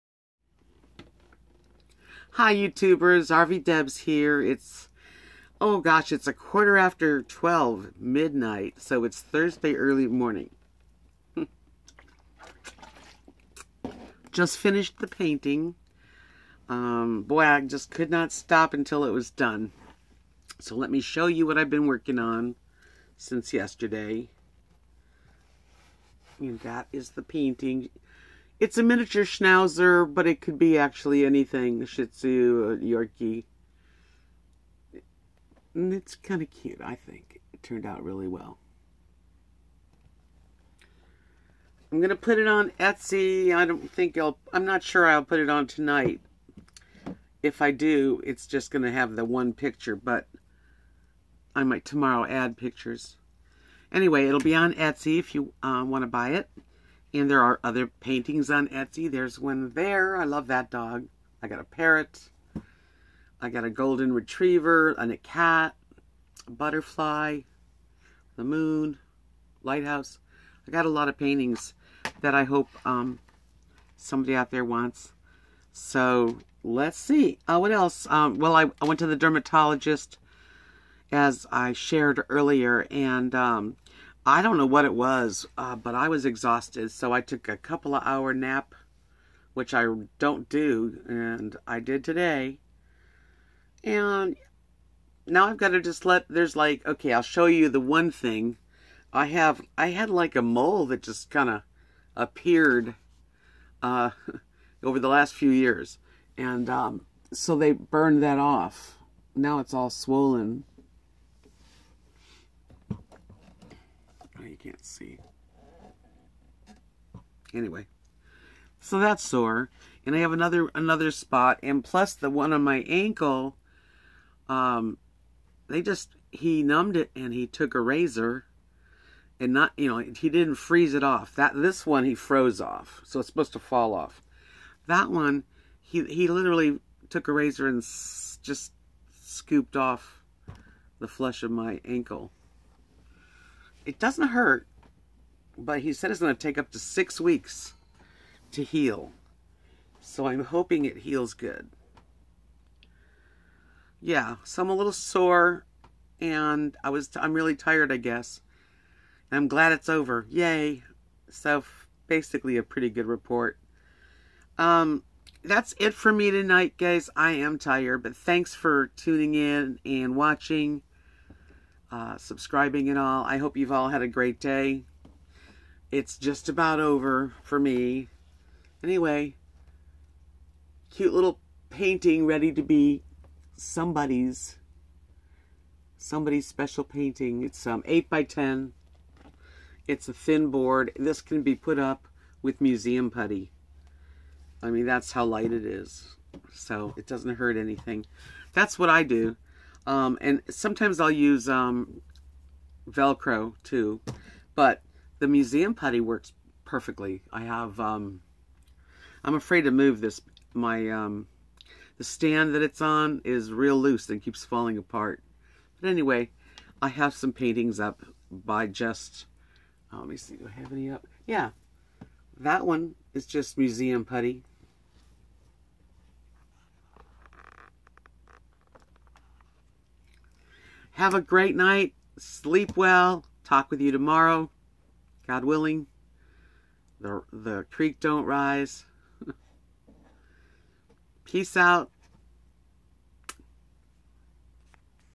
Midnight showing my latest doggy painting
This video is showing my latest painting which is an 8 by 10 painting of a miniature schnauzer I'll be listing it on Etsy here's the link to my Etsy shop